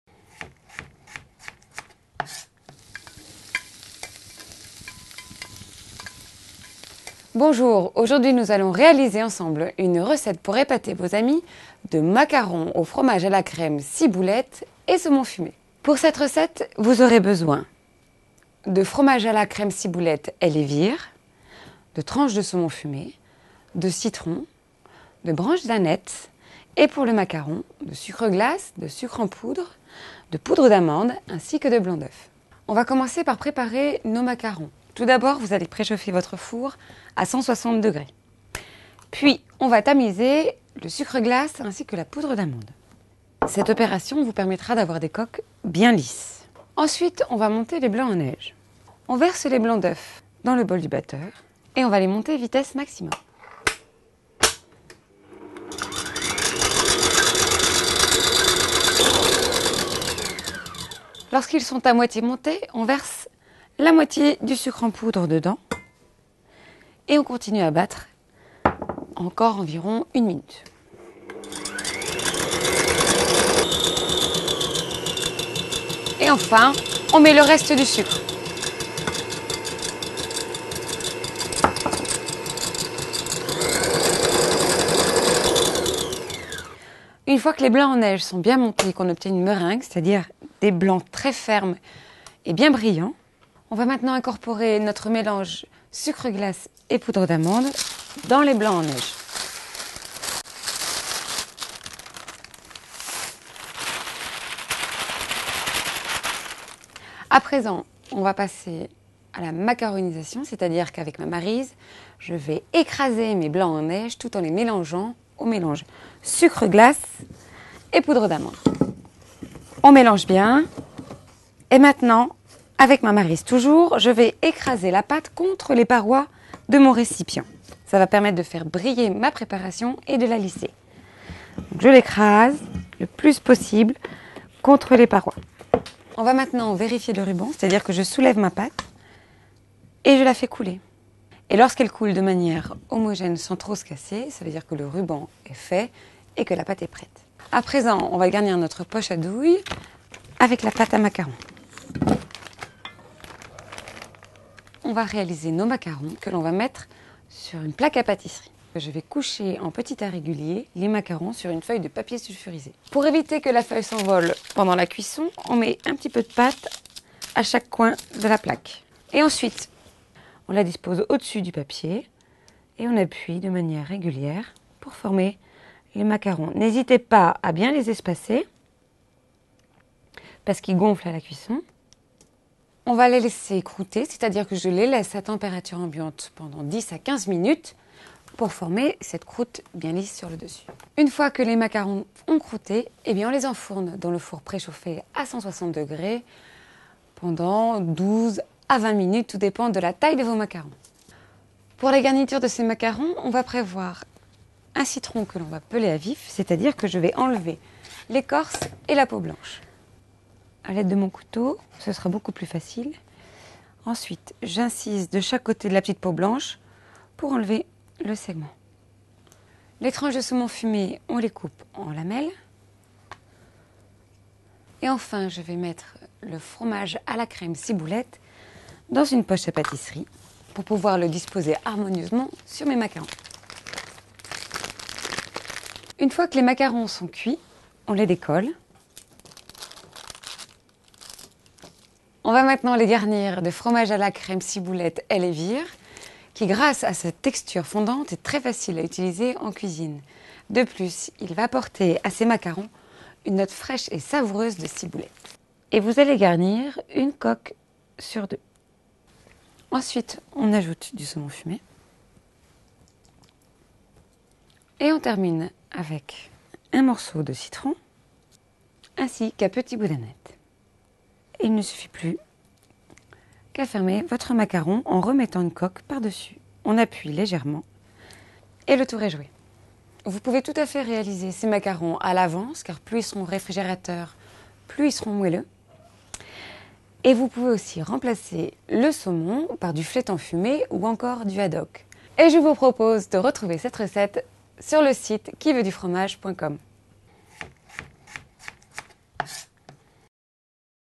Recette de macaron au saumon fumé et Fromage à la Crème de ciboulette
Recette en vidéo de L'atelier des Chefs de macarons salés garnis de Fromage à la Crème ciboulette Elle&vire, saumon fumé, citron frais et aneth.

Quantités pour 6 personnes :

    Fromage à la Crème ciboulette Elle&Vire : 150 g
    Sucre glace : 350 g
    Poudre d'amande : 250 g
    Blanc(s) d'oeuf(s) : 215 g
    Sucre semoule : 150 g
    Tranche(s) de saumon fumé de Norvège : 6 pc
    Citron(s) jaune(s) : 1 pc
    Aneth : 3 branche(s)

Retrouvez cette recette en vidéo en cliquant ainsi que toutes les vidéos des Chefs de L'atelier des Chefs